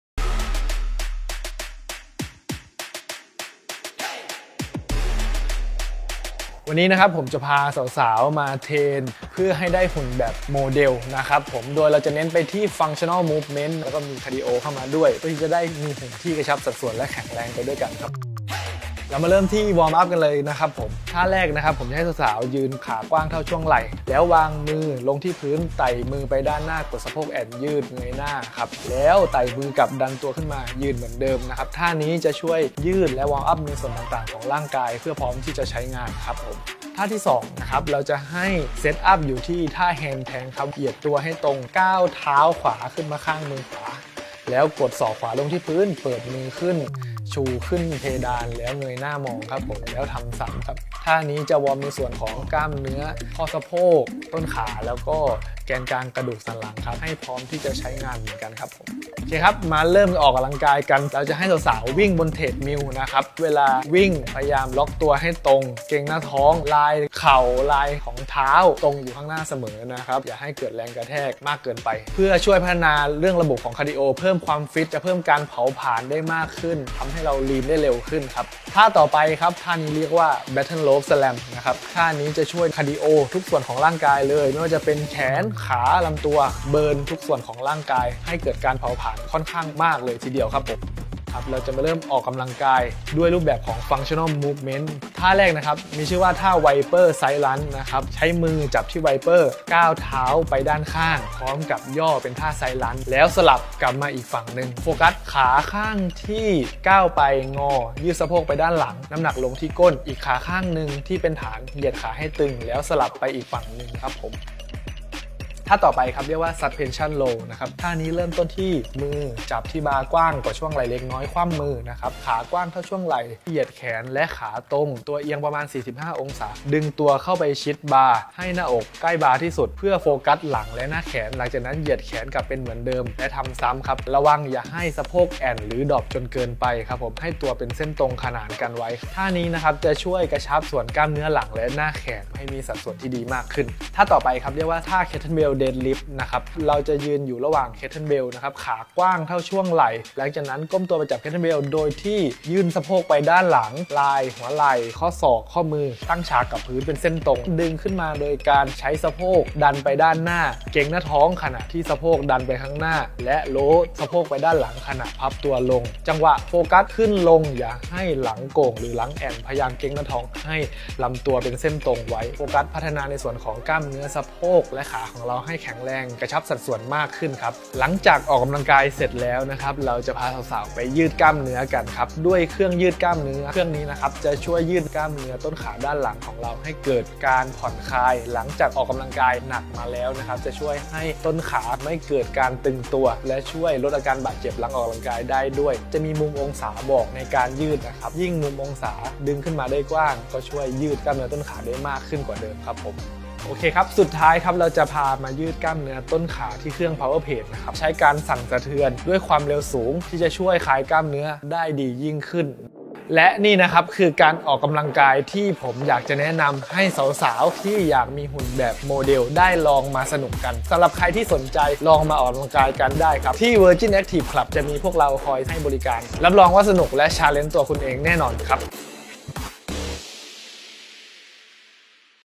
Workout Like a Model | 9 Steps Workout with VirginActive
ออกกำลังกายไม่ซ้ำ แต่ได้ทำทุกส่วนให้ฟิตเหมือนนางแบบที่หุ่นดีๆ สะกิดถามเทรนเนอร์เลย!

เขาจะเน้นที่ Functional Movement และ Cardio เป็นหลัก วอร์มอัพกันก่อนเพื่อยืดกล้ามเนื้อส่วนต่างๆ ของร่างกายให้พร้อมใช้งาน แล้วไปต่อที่การคาดิโอ วิ่งบนลู่ และ Battle Rope Slam เสร็จแล้วตามมาติดๆ ด้วย Viper Side Lunge กับ Suspension Roll และ Kettlebell Deadlift เป็นการออกกำลังกายแบบ Functional Movement กระชับทุกสัดส่วนทั่วร่างกาย ปิดท้ายด้วยการผ่อนคลายหลังออกกำลังกายด้วยเครื่องยืดกล้ามเนื้อและเครื่อง Power Plate ลดอาการบาดเจ็บหลังออกกำลังกาย
ที่ Virgin Active Thailand เขามีจัดเต็มอุปกรณ์ออกกำลังกายในทุกรูปแบบเอาไว้ให้ใช้ บางเครื่องเพิ่งเคยได้ลองที่นี่ ใช้ไม่เป็นเทรนเนอร์สอนได้ เอนจอยกับการได้ลองอะไรใหม่ๆ แบบนี้ แล้วการออกกำลังกายก็จะให้ความสนุกกับเราเอง